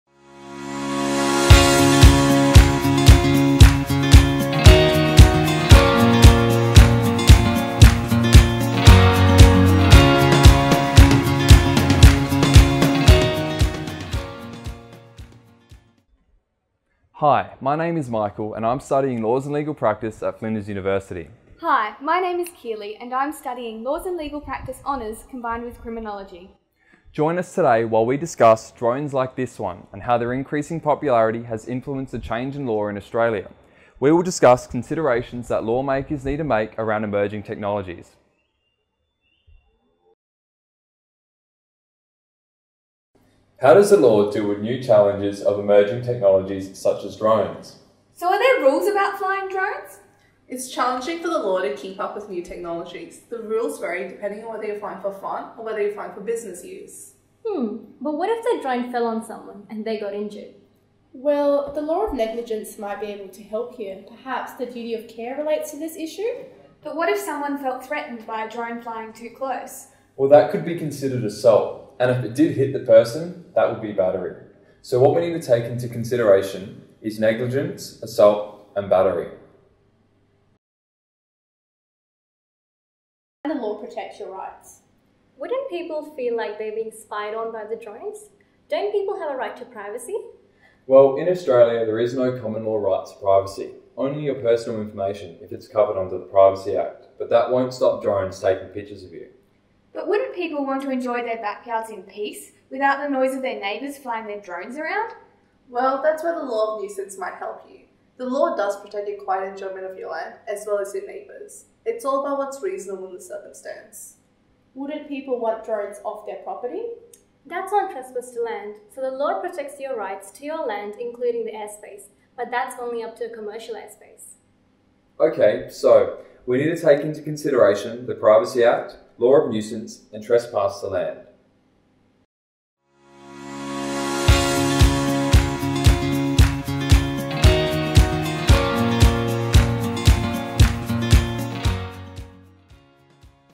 Flinders VR - Law
Welcome to the Flinders University virtual reality experience. Learn more about studying Law at Flinders and the degree options available. Join a number of Flinders Law students discussing drones and how their increase in popularity has influenced a change in law in Australia. The students discuss considerations that law makers need to make around emerging technologies.